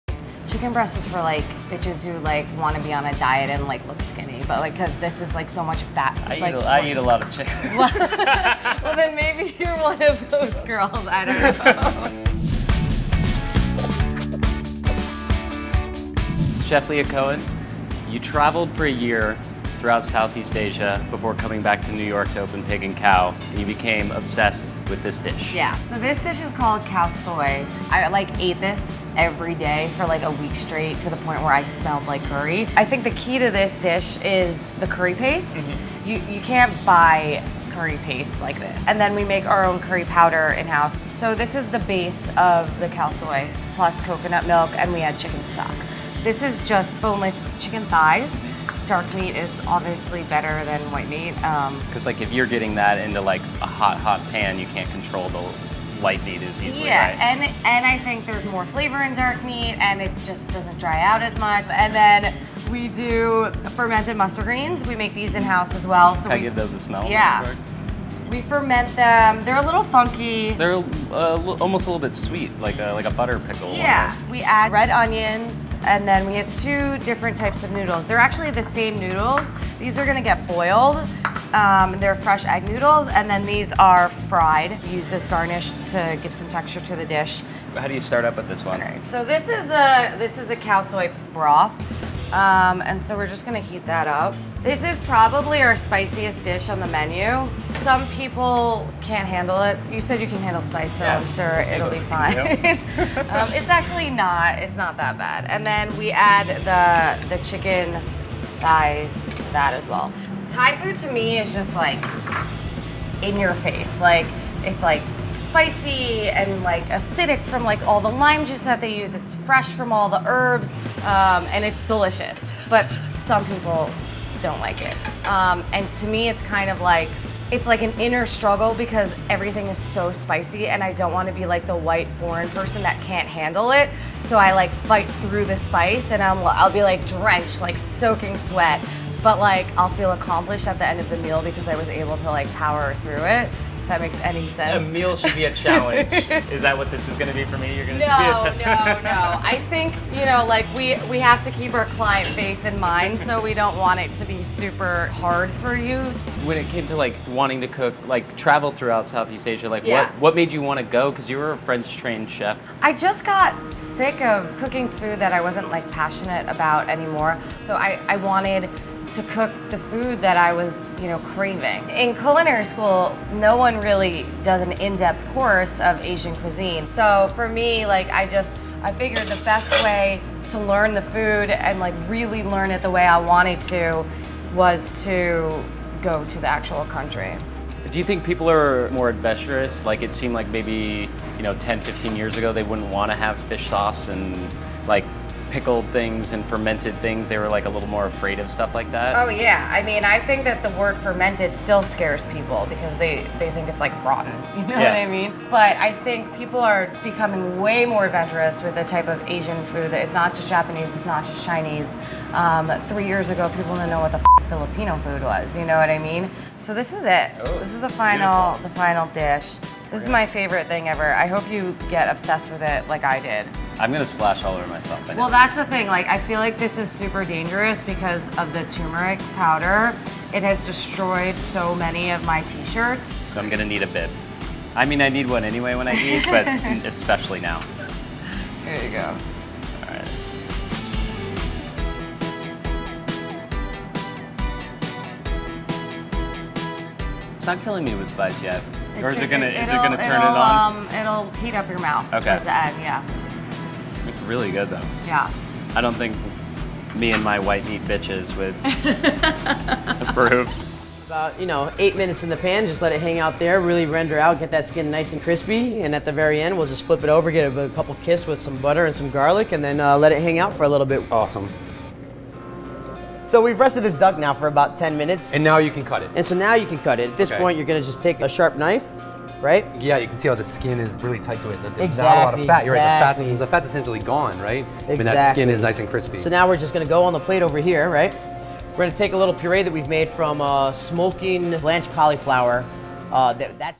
Making Spicy Thai Noodles with Chef Leah Cohen | Let's Get Fat
Chef Leah Cohen of Pig and Khao on New York’s Lower East Side fell in love with the spicy, Thai noodle dish Khao Soi when she traveled throughout Southeast Asia a few years ago. She made the dish for Playboy senior editor Jeremy Repanich and questioned why he’d ever eat white meat chicken instead of the more flavorful and tender dark meat.

Executive Producer: Brian Berkowitz
Producer: Jeremy Repanich
Host: Jeremy Repanich